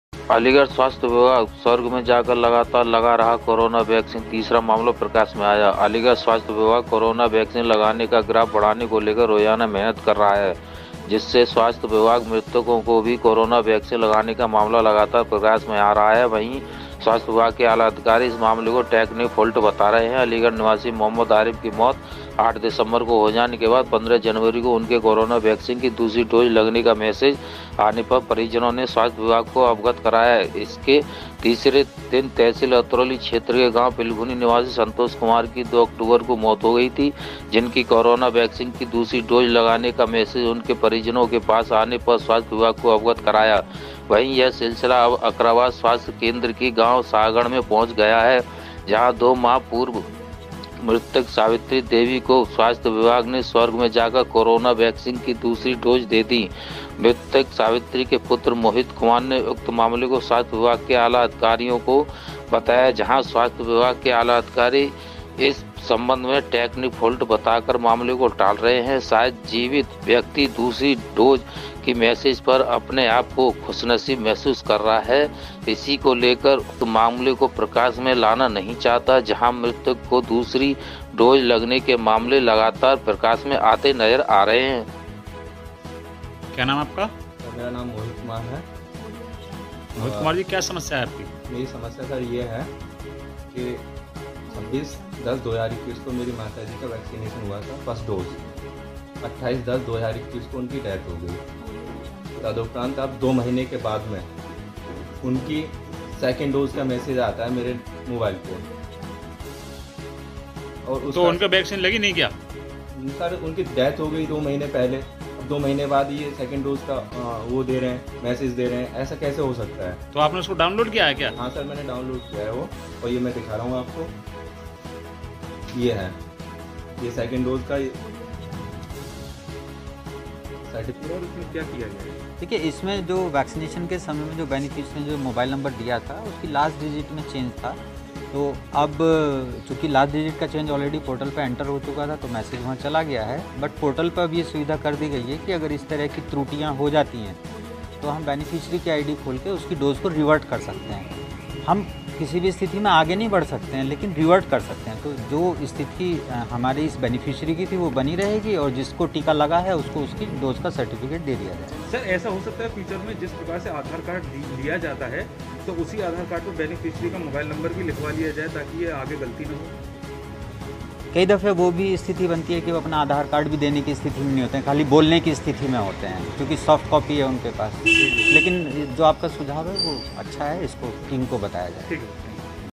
अलीगढ़ स्वास्थ्य विभाग स्वर्ग में जाकर लगा रहा लगातार कोरोना वेक्सीन, तीसरा मामला प्रकार में आया,
स्लग--- अलीगढ़ स्वास्थ्य विभाग स्वर्ग में जाकर लगा रहा लगातार कोरोना वेक्सीन, तीसरा मामला प्रकार में आया,

एंकर- अलीगढ़ स्वास्थ्य विभाग कोरोना वैक्सीन लगाने का ग्राफ बढ़ाने को लेकर रोजाना मेहनत कर रहा है जिससे स्वास्थ्य विभाग मृतकों के भी कोरोना वैक्सीन लगाने का मामला लगातार प्रकाश में आ रहा है वहीं स्वास्थ्य विभाग के आला अधिकारी इस मामले को टेक्निकल फॉल्ट बता रहे हैं अलीगढ़ निवासी मोहम्मद आरिफ की मौत 8 दिसंबर को हो जाने के बाद 15 जनवरी को उनके कोरोना वैक्सीन की दूसरी डोज लगने का मैसेज आने पर परिजनों ने स्वास्थ्य विभाग को अवगत कराया इसके तीसरे दिन तहसील अतरौली क्षेत्र के गांव पिलखुनी निवासी संतोष कुमार कि 2 अक्टूबर को मौत हो गई थी जिनकी कोरोना वैक्सीन की दूसरी डोज लगने का मैसेज उसके परिजनों के पास आने पर स्वास्थ्य विभाग को अवगत कराया वहीं यह सिलसिला अब अकराबाद स्वास्थ्य केंद्र की गांव शाहगढ़ में पहुंच गया जहां 2 माह पूर्व मृतक सावित्री देवी को स्वास्थ्य विभाग ने स्वर्ग में जाकर कोरोना वैक्सीन की दूसरी डोज दे दी, मृतक सावित्री के पुत्र मोहित कुमार ने उक्त मामले को स्वास्थ्य विभाग के आला अधिकारियों को बताया जहां स्वास्थ्य विभाग की आला अधिकारी इस संबंध में टेक्निकल फाल्ट बताकर मामले को टाल  रहे हैं शायद जीवित व्यक्ति दूसरी दूज की मैसेज पर अपने आप को खुशनसीब महसूस कर रहा है इसी को लेकर वह इस उक्त मामले को प्रकाश में लाना नहीं चाहता जहां मृतक को दूसरी दूज लगने के मामले लगातार प्रकाश में आते नजर आ रहे हैं

बाइट मृतक महिला का पुत्र मोहित कुमार

बाइट सीएमओ अलीगढ़